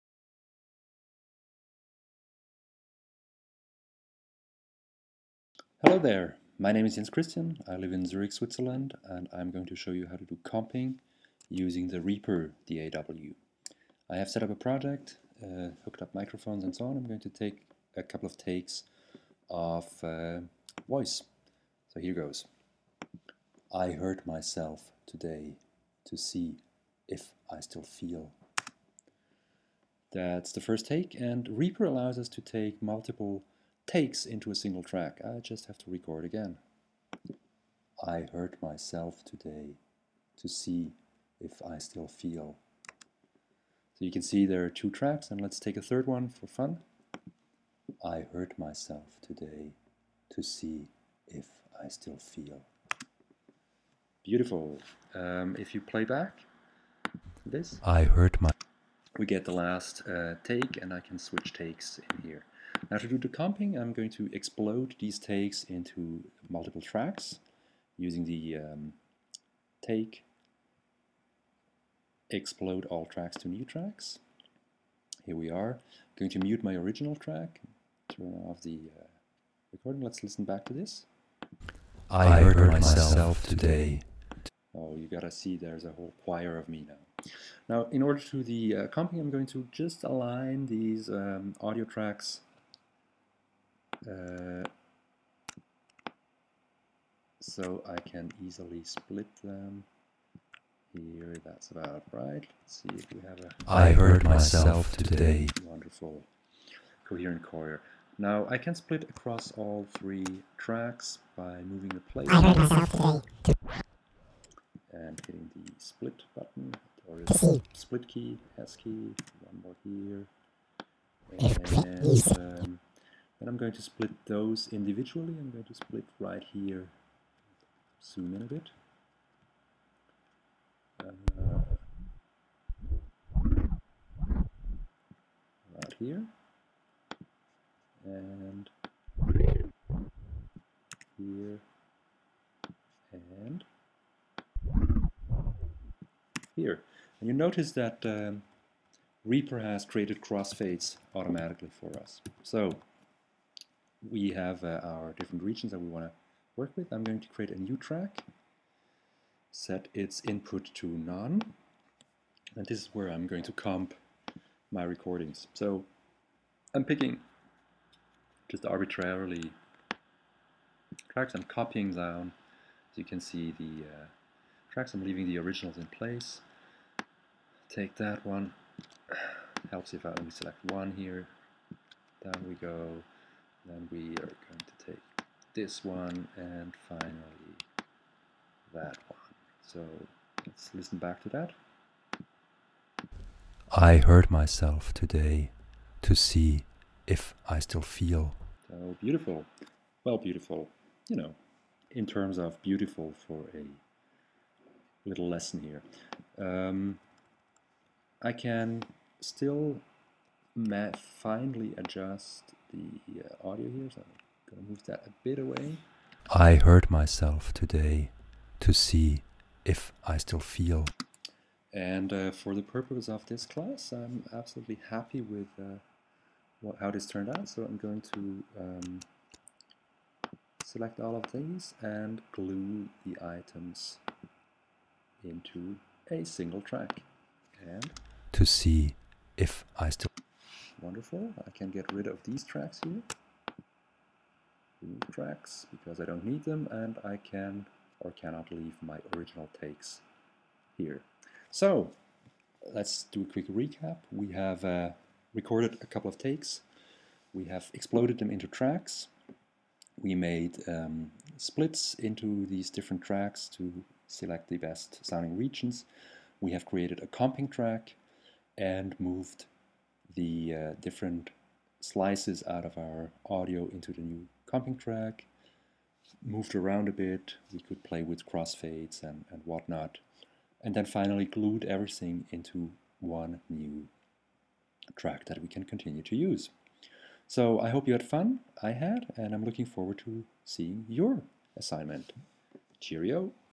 IMPMOOC - Assignment 2 - Comping Audio tracks with Reaper
This video is for assignment #2 of the Coursera "Introduction to Music Production" Course.

It shows one way of comping multiple takes of an audio track into a new track